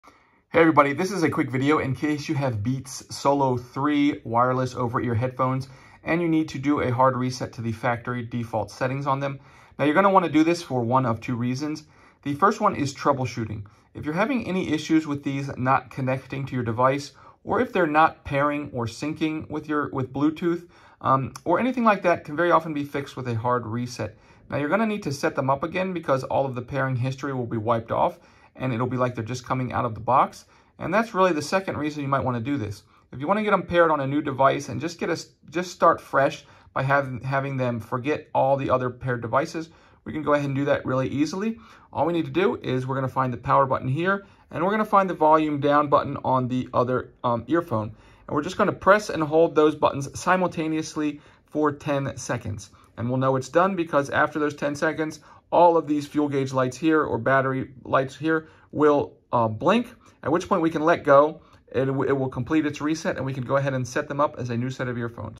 Hard Reset Beats Solo 3 Earphones
How to troubleshoot or setup starting with a force hard reset a Beats Solo 3 wireless bluetooth Headphones to factory default settings if it is not working or not connecting or if it is frozen or not responding, the factory reset is a good troubleshooting step. You can also restore the factory settings to setup your device as new. Found in the Beats Solo 3 Headphones user manual.